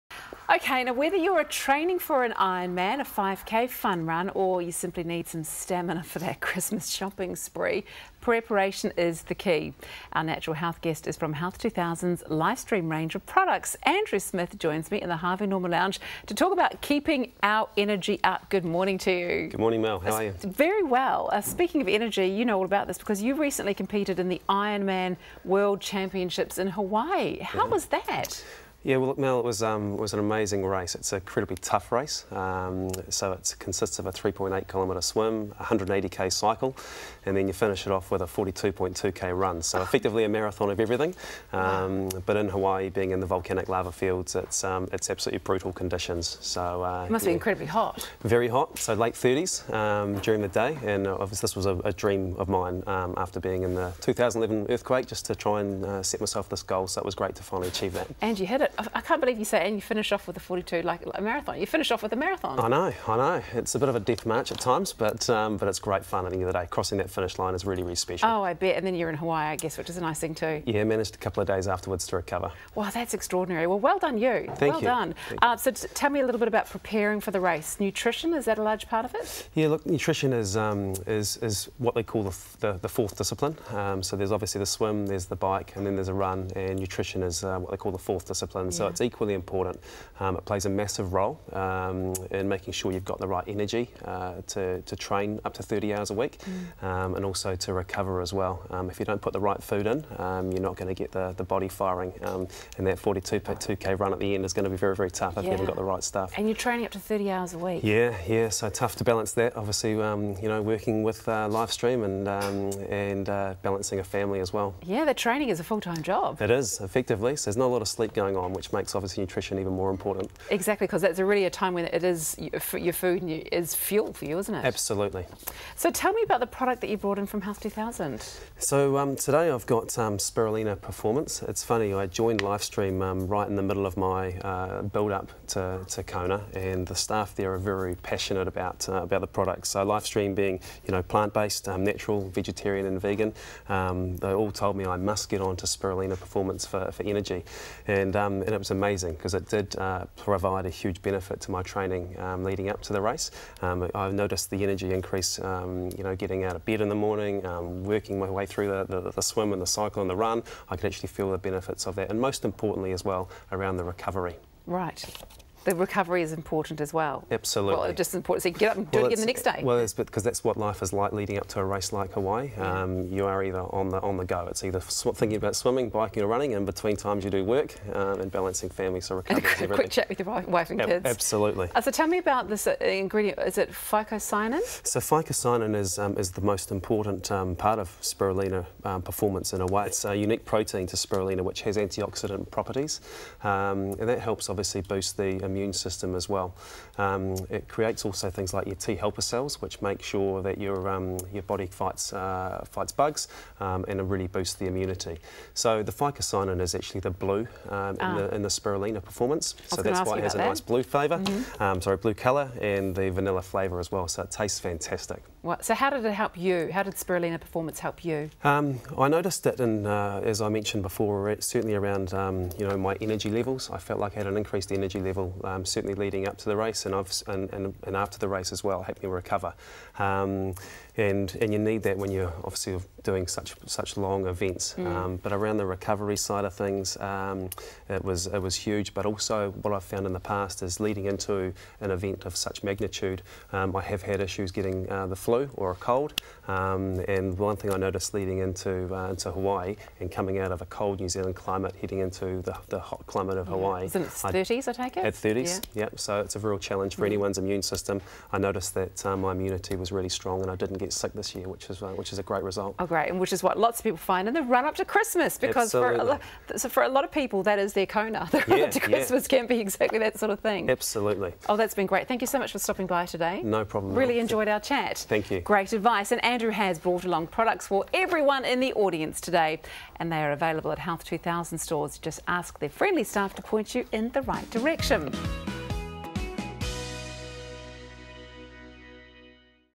Andrew Smith joins us to talk about keeping your energy up
Whether you’re training for an Ironman, a 5k fun run or simply need some stamina for that Christmas shopping, Andrew Smith recommends Lifestream's Spirulina Performance from Health 2000.